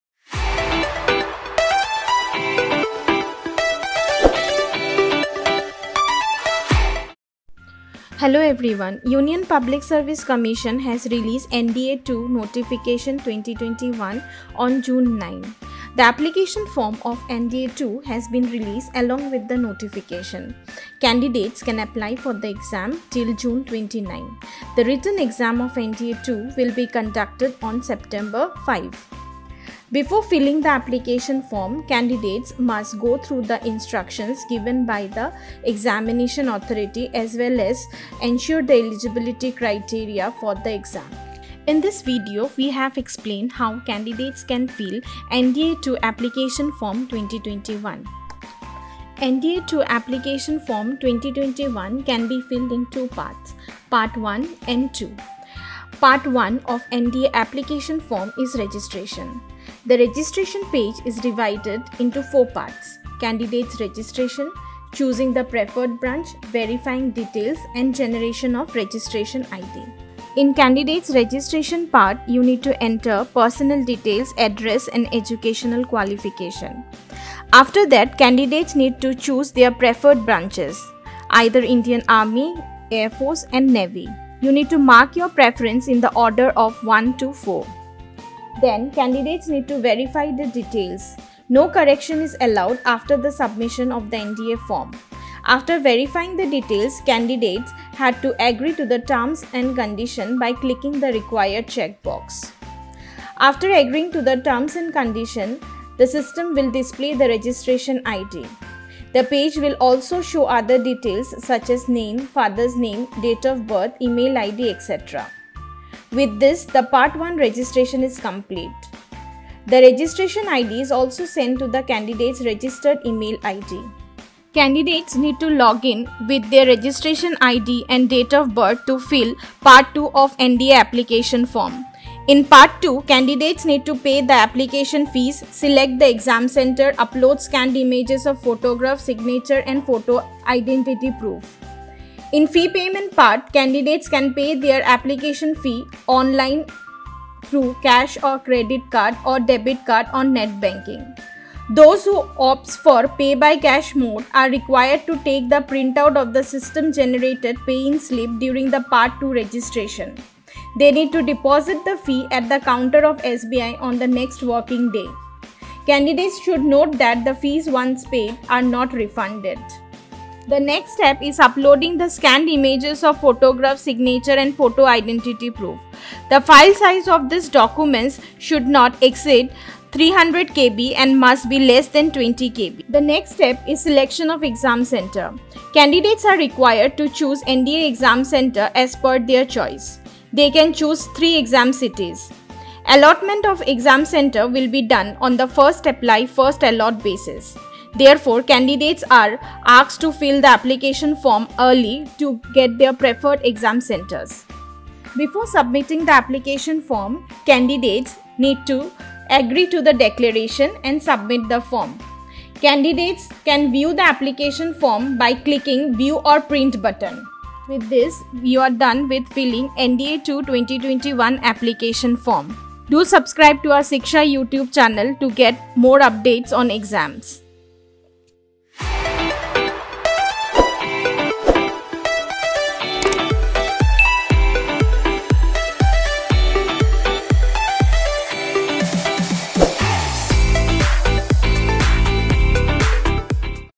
How to fill NDA (2) 2021 Application Form? Check Step-Wise Process
NDA (II) application form 2021 was released on June 9. The last date to fill the application form is June 29. The last date to pay the application fees offline is June 28, 2021. The online fee payment can be done till July 29, 2021. Watch this video on how to fill NDA (2) 2021 Application Form to register for the exam smoothly.

Now ask your queries on Whatapp by sending a Hi on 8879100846